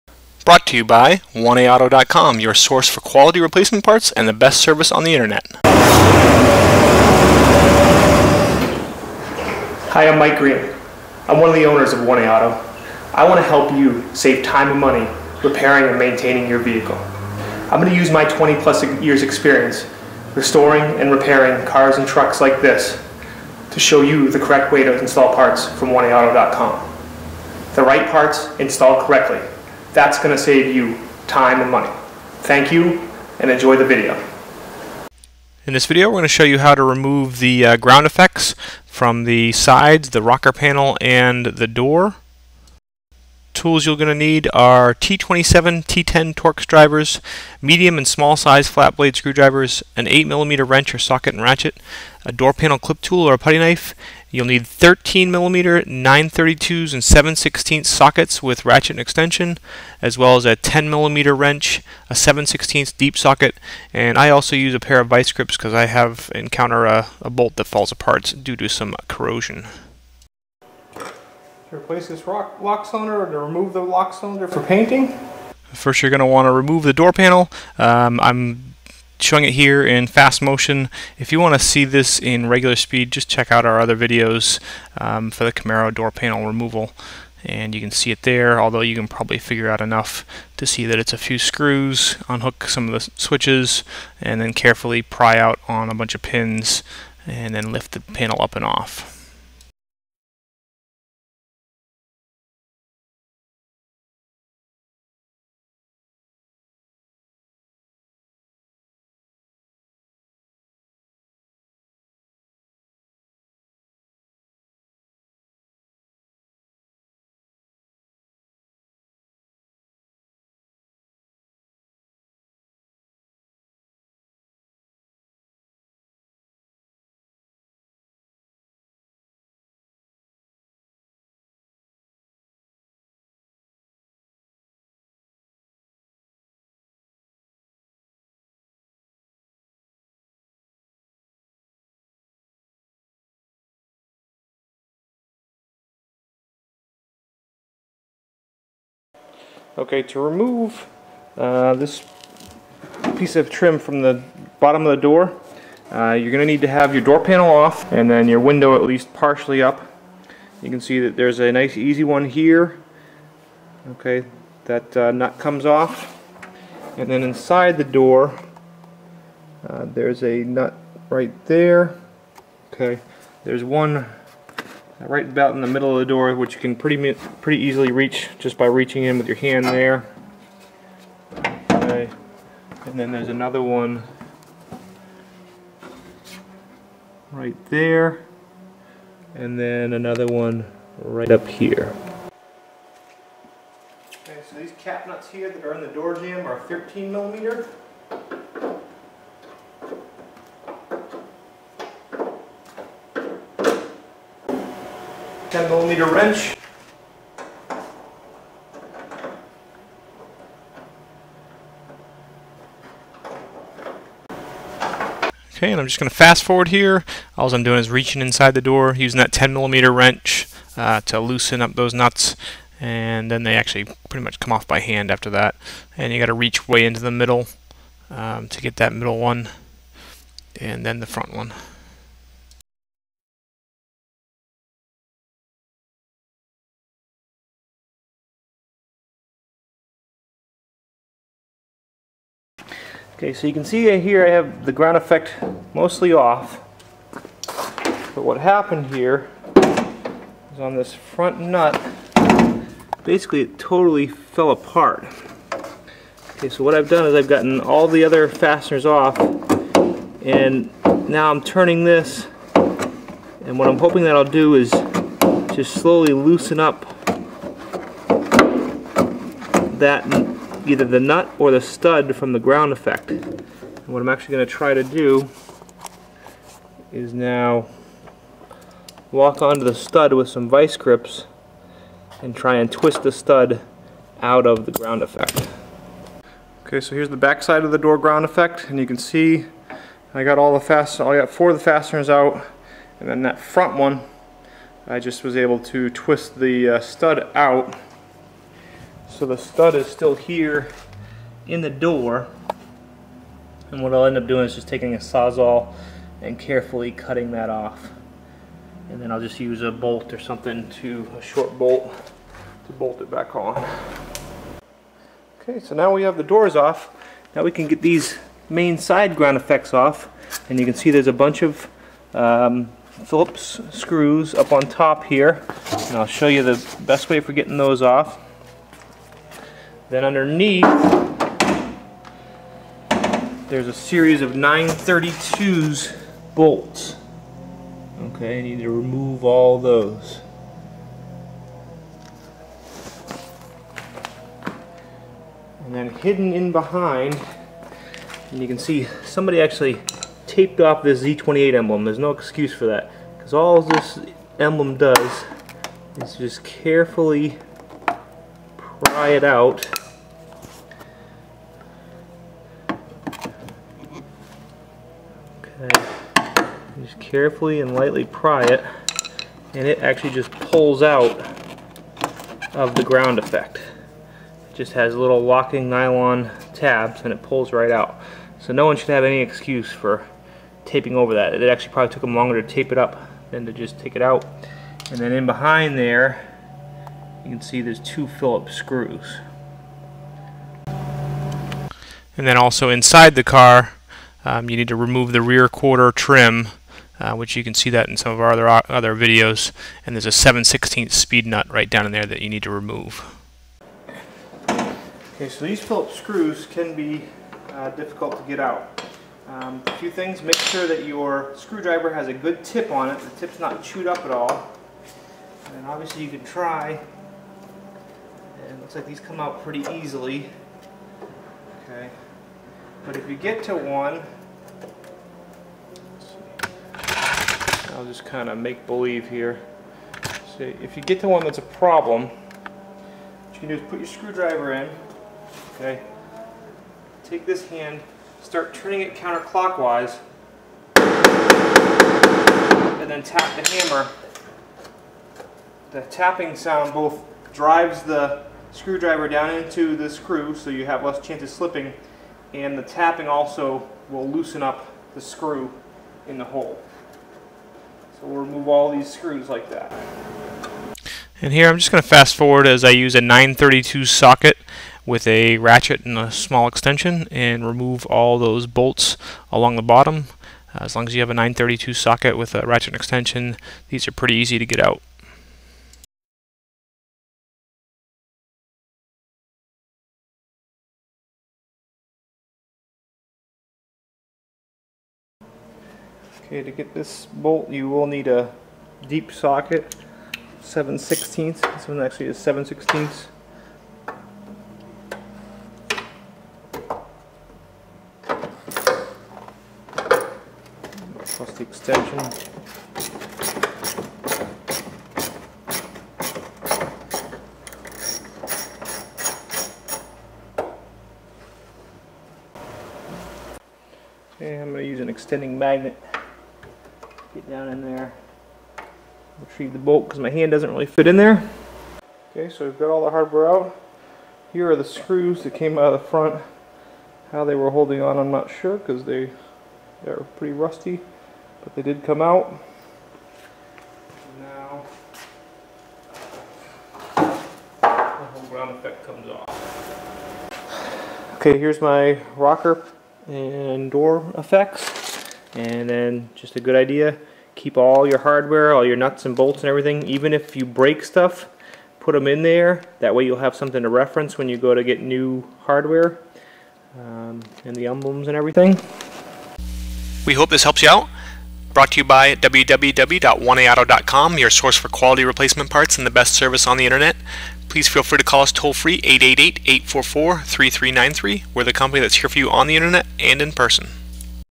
How to Replace Door and Rocker Panel 82-92 Camaro IROC-Z
In this video, 1A Auto shows you how to repair, install, fix, change or replace the door and rocker panel ground effects on a Camaro Iroc-Z. This video is applicable to the Chevy Camaro, years 82, 83, 84, 85, 86, 87, 88, 89, 90, 91, and 92.